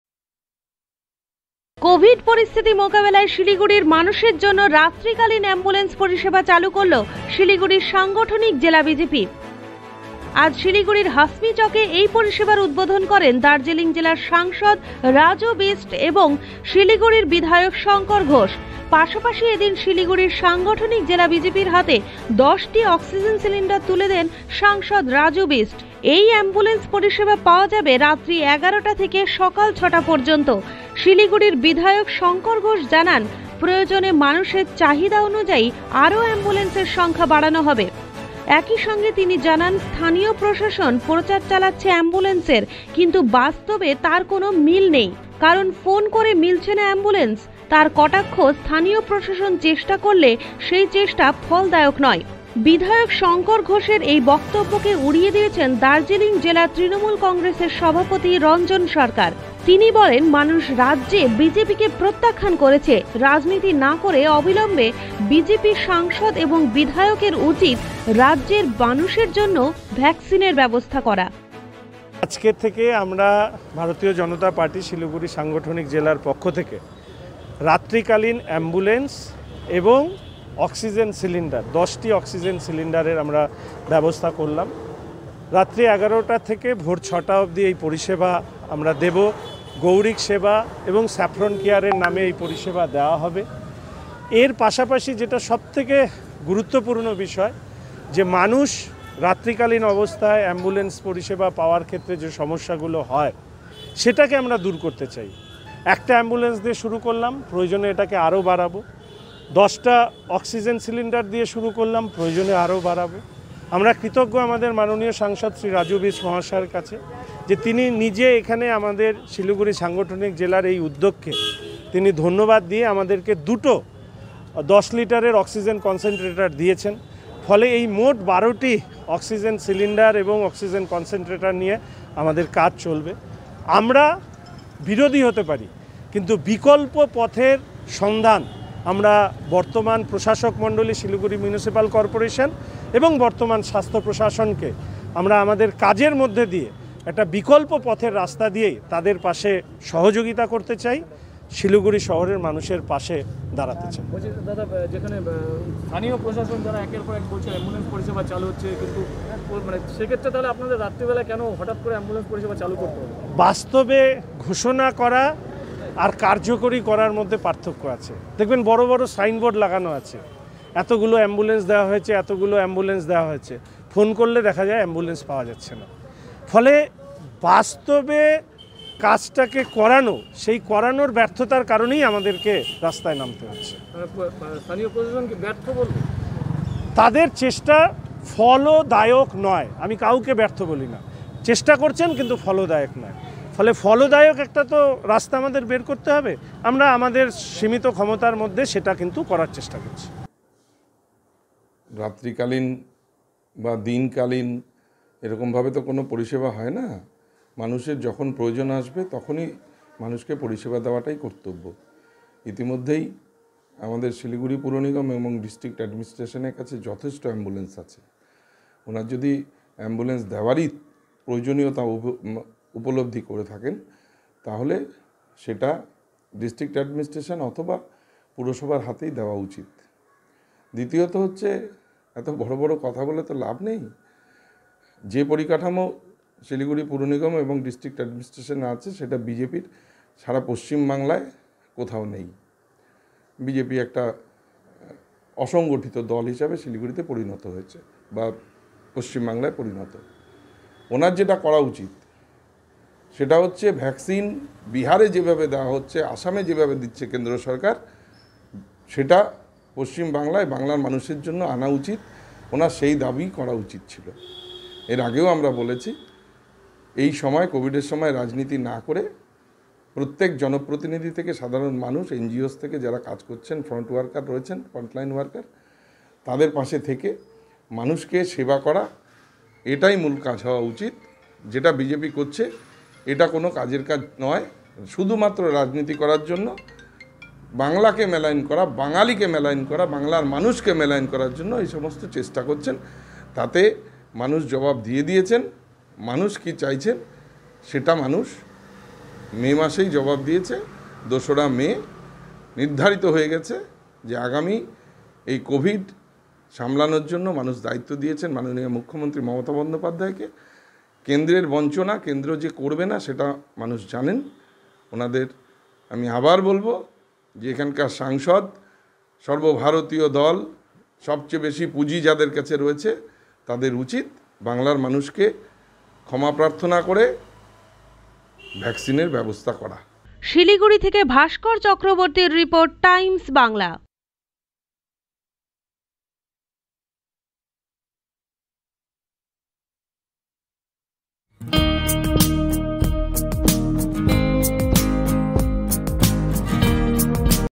শিলিগুড়ির মানুষের জন্য রাত্রিকালীন অ্যাম্বুলেন্স পরিষেবা চালু করল শিলিগুড়ি সাংগঠনিক জেলা বিজেপি
কোভিড পরিস্থিতি মোকাবেলায় শিলিগুড়ির মানুষের জন্য রাত্রিকালীন অ্যাম্বুলেন্স পরিষেবা চালু করল শিলিগুড়ি সাংগঠনিক জেলা বিজেপি